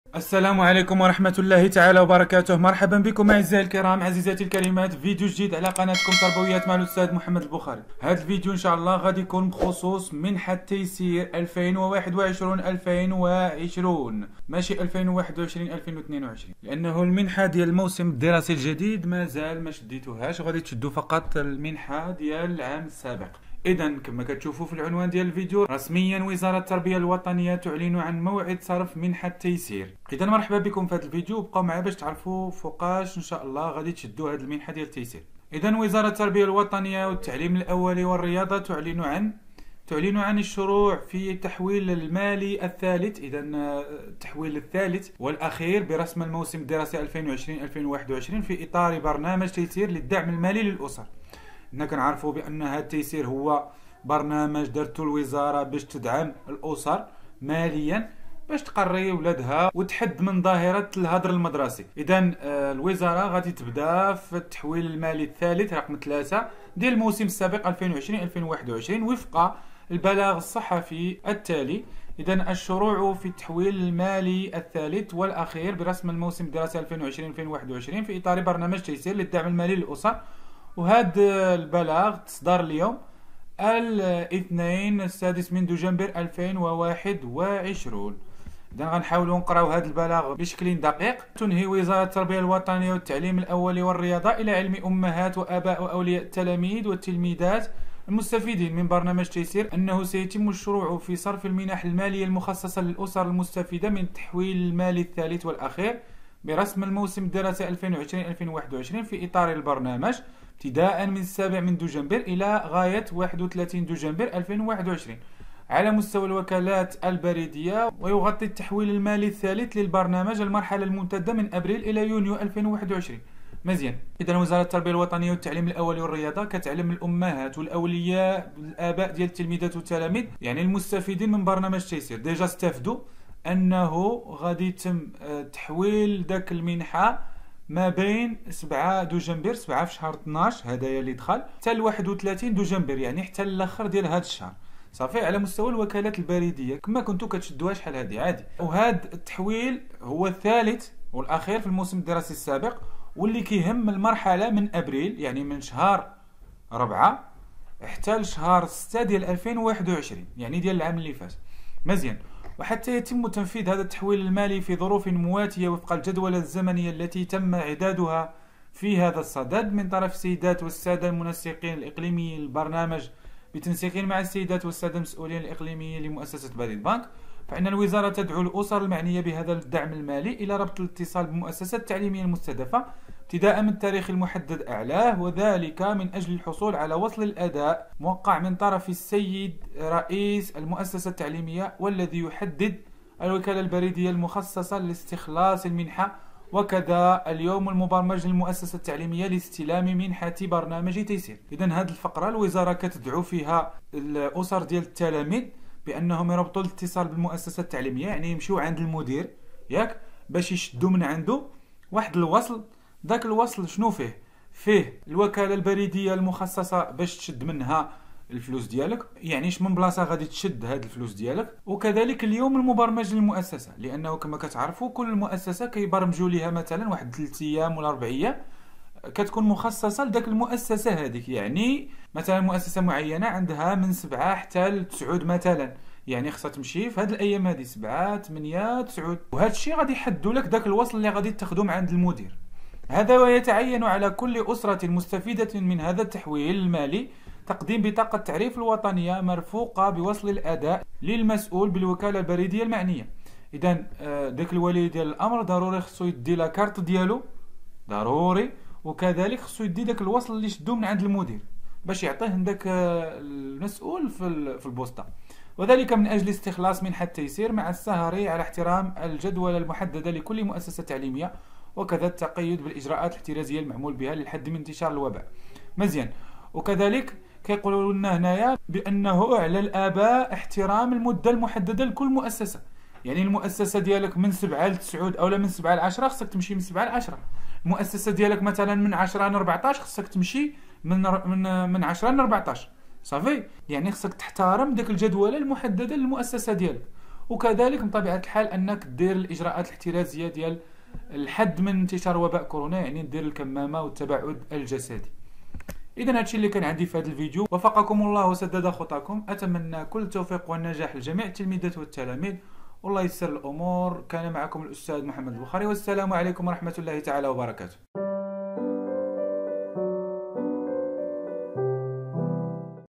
عاااجل !! وزارة التربية الوطنية تعلن عن موعد صرف منحة برنامج "تيسير" ؟
تلقيح جميع التلاميذ سيضمن دخول مدرسي حضوري.. كيف ذلك؟ محمد أضرضور يوضح نمط التعليم (حضوري ام عن بعد)